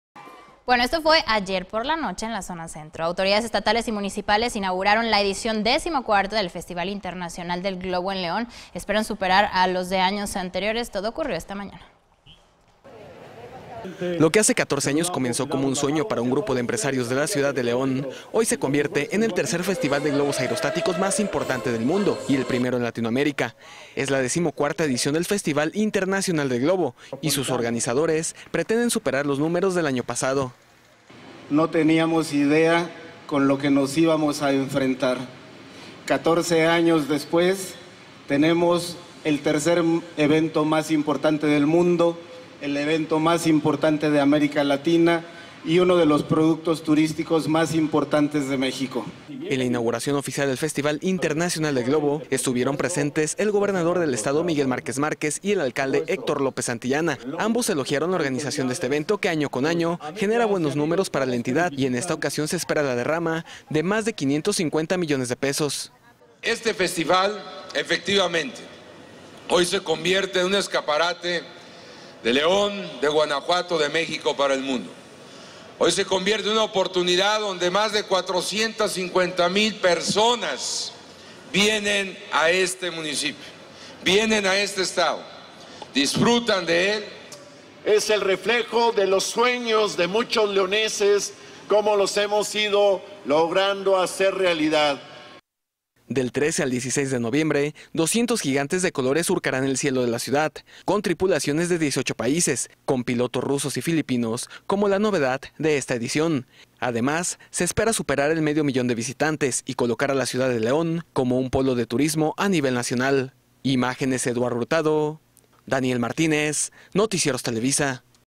Inauguración del Festival Internacional del Globo
Autoridades municipales y estatales estuvieron presentes en la inauguración del FIG 2015